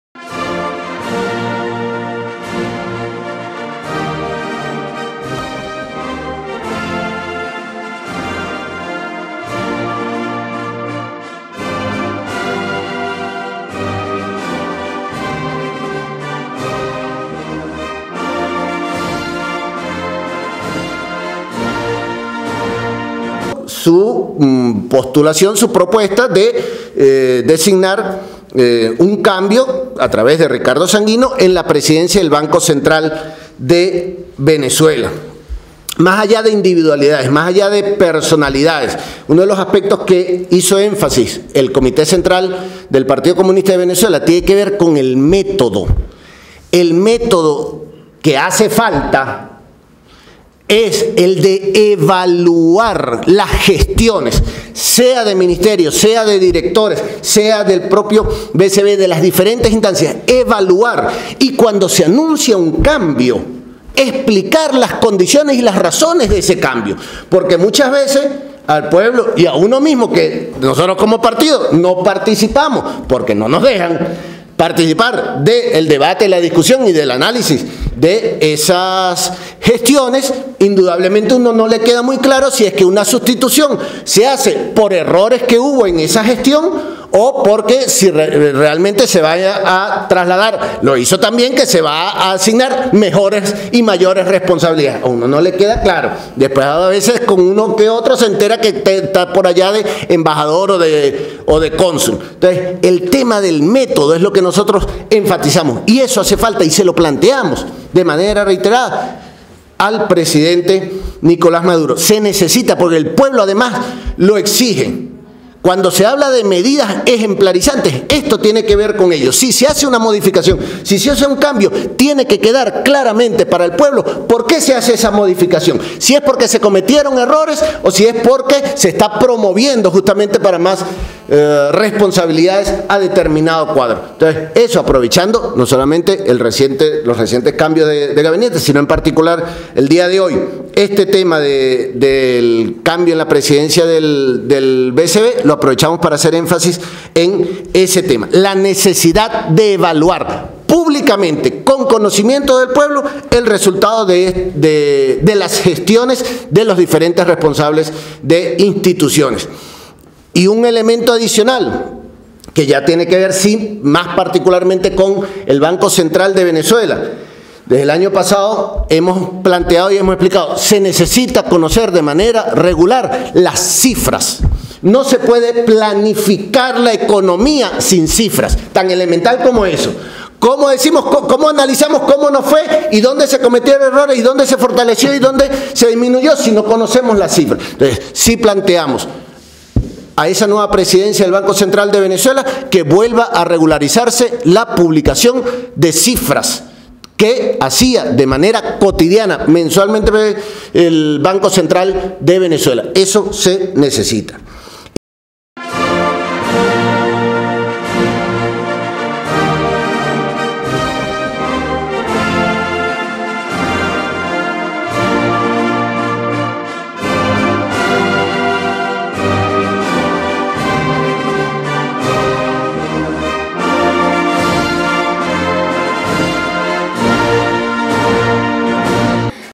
PCV: Cambios de Gabinete requieren una evaluación pública de la gestión.
Pide que BCV publique las cifras de la economía. No se puede planificar sin conocerlas.

Caracas, 23 ene. 2017, Tribuna Popular TP.- El Buró Político del Partido Comunista de Venezuela (PCV) señaló la necesidad que ante cualquier cambio de gabinete u otras instituciones del Estado, se realice una evaluación pública de la gestión de los funcionarios salientes y llamó al nuevo presidente del Banco Central de Venezuela (BCV) a publicar periódicamente las cifras de la economía venezolana.